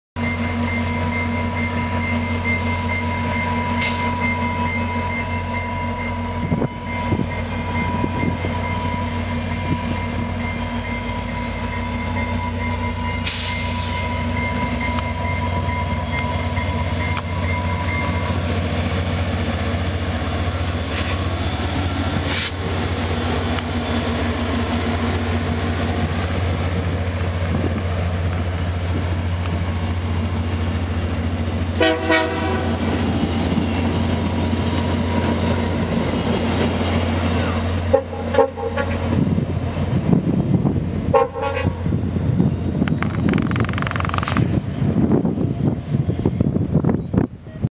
VIA 54 Leaves Brockville
F40PH 6439 leads train 54 toward Ottawa on May 31st, 2013.